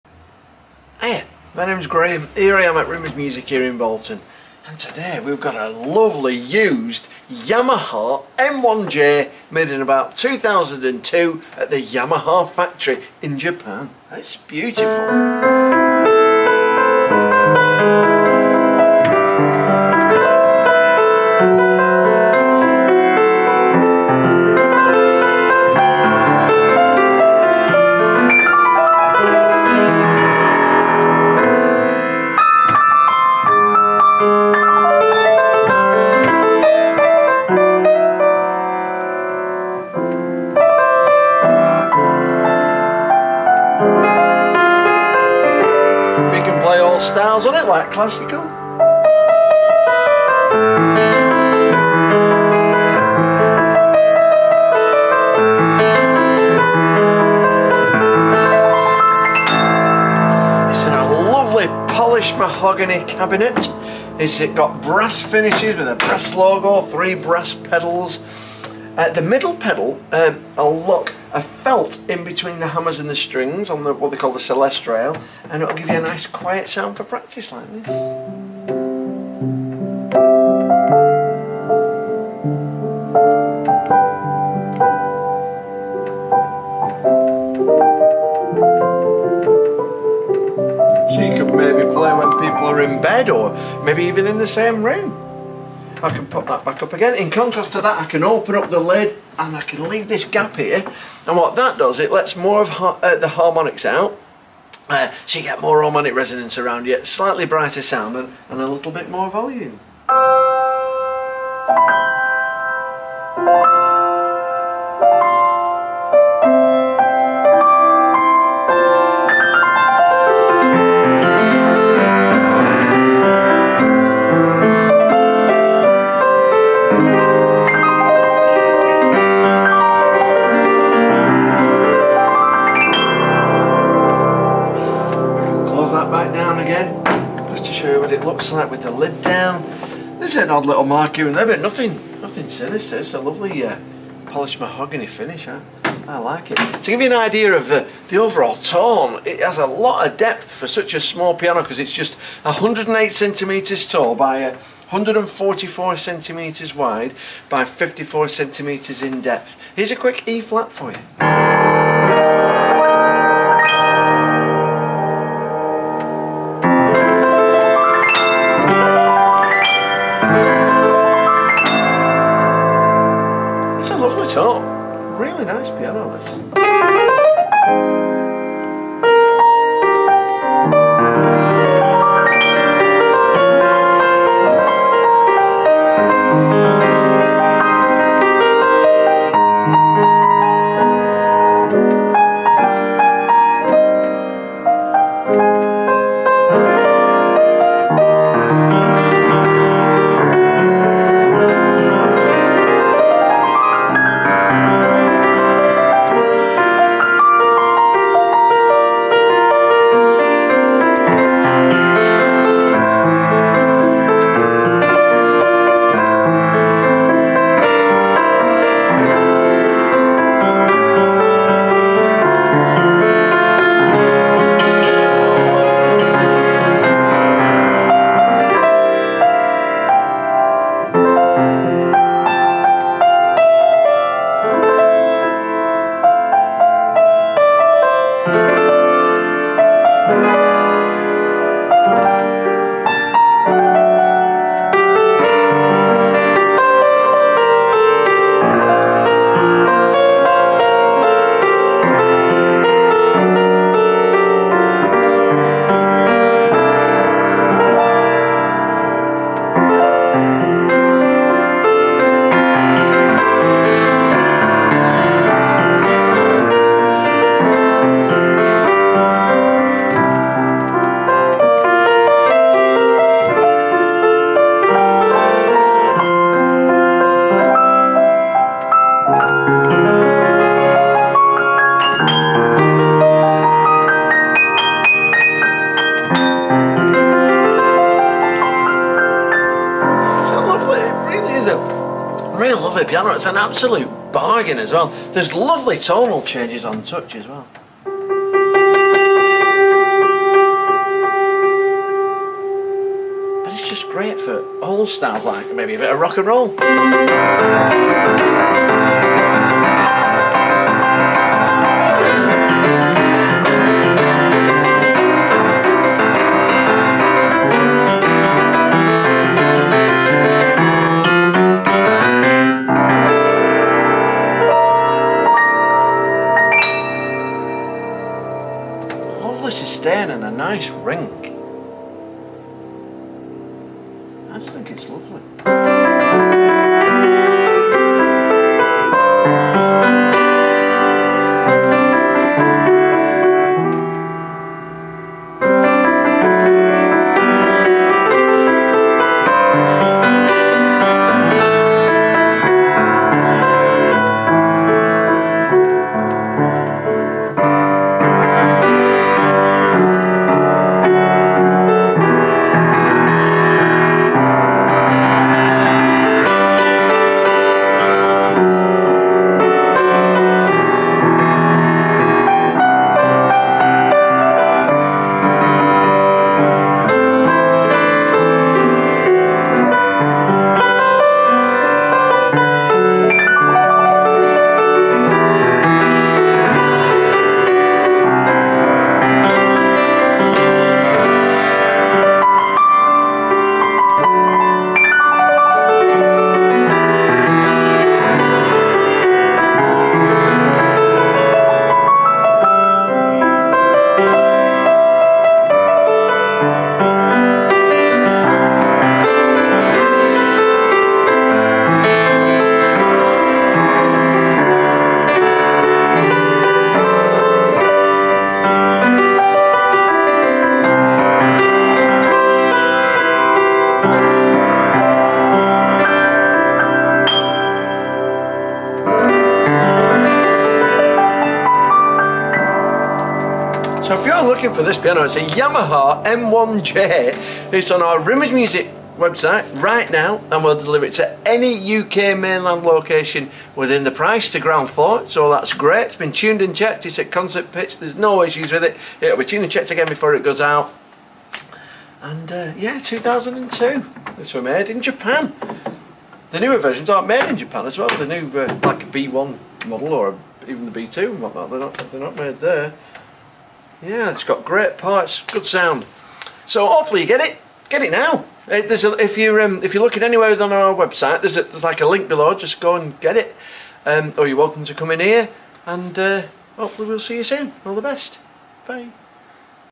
Used Yamaha M1J Upright Acoustic Piano Demonstratin & Review | Piano For Sale At Rimmers Music
Used Yamaha M1J Upright Piano | Polished Mahogany | Bolton Store

    3 Months Parts & Labour Guarantee
    Pedals: 3 Pedals - Una Chorda | Practise Pedal (Known As 'Celeste Pedal')
    Sustain Pedal
    Manufactured In Japan Approximately 2002